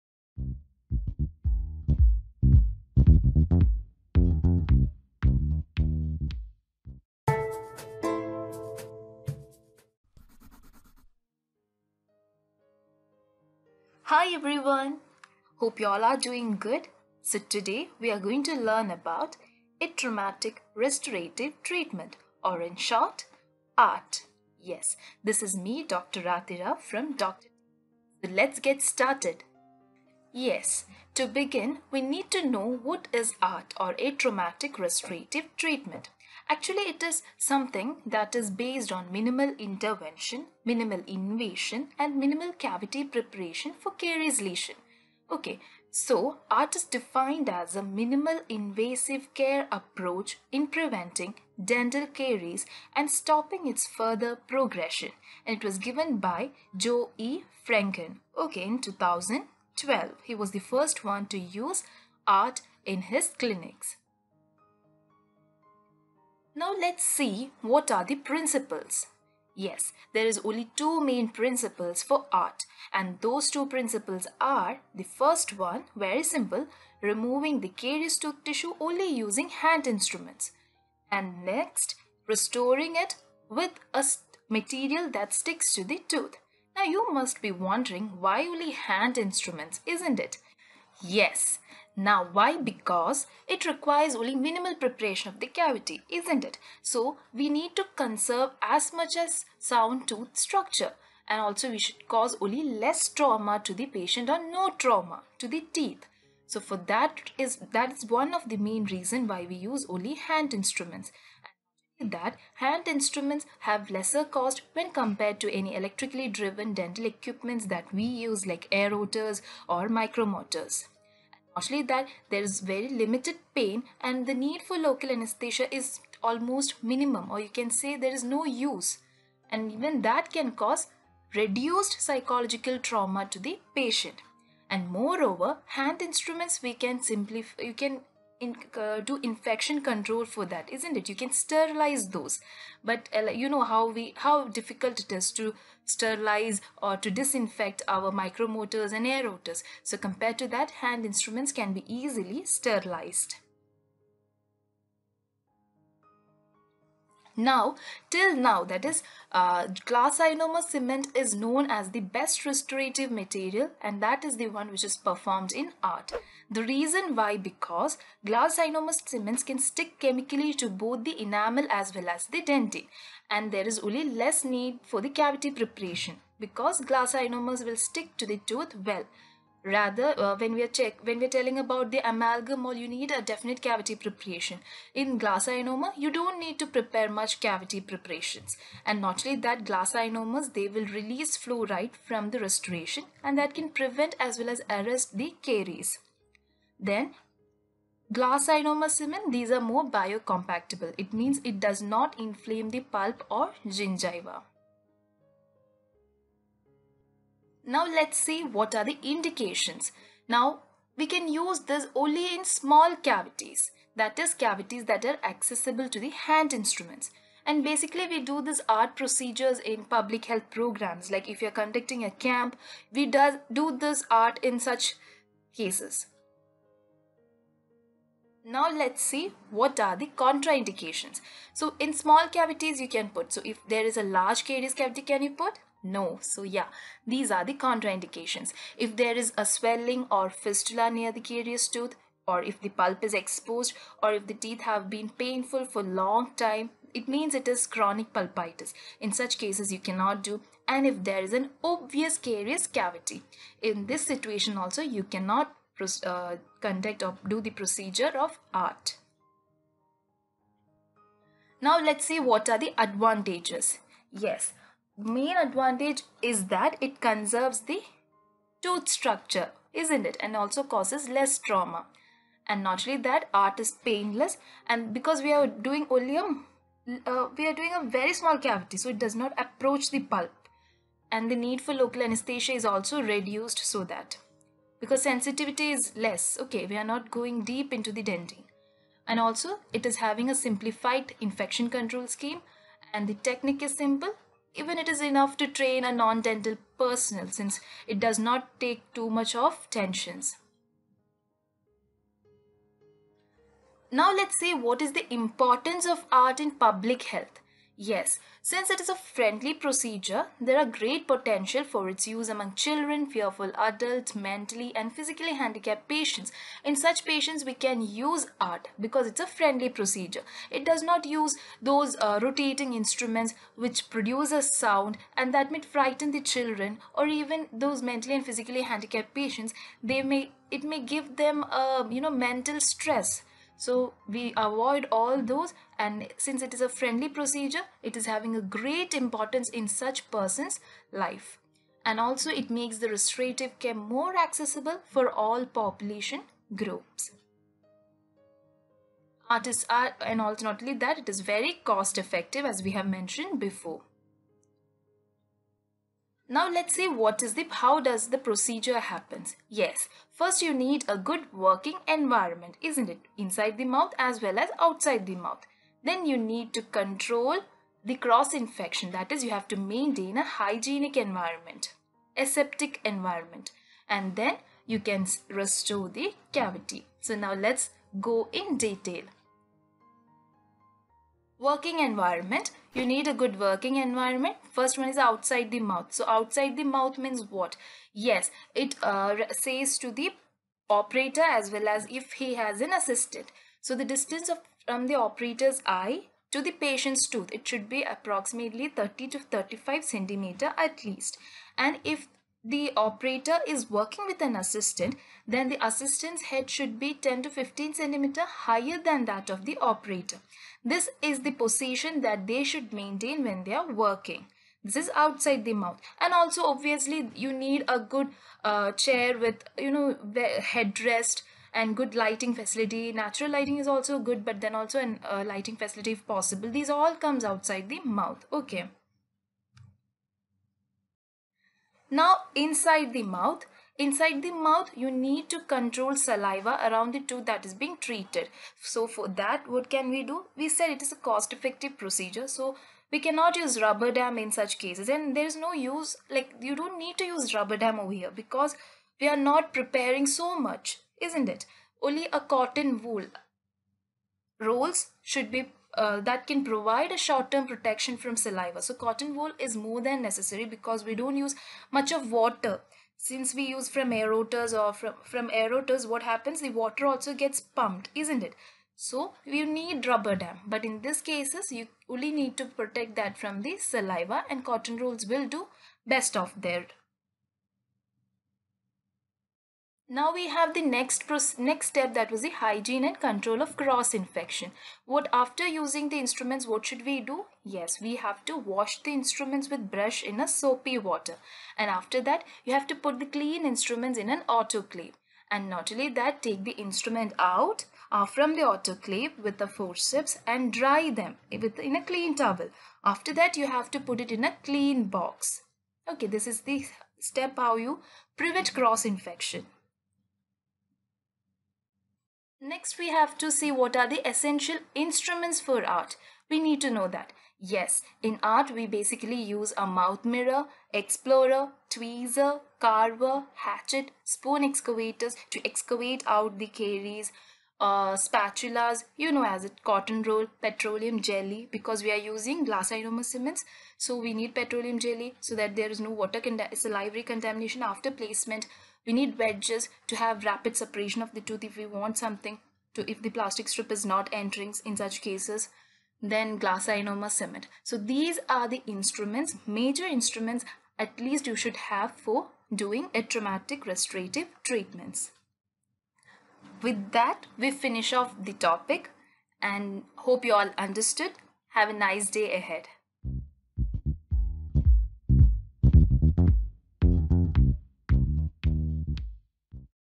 DENTAL PROCEDURES | The art behind ART | Atraumatic Restorative Treatment
Extremely helpful for dental exam preparation, NEET-MDS preparation.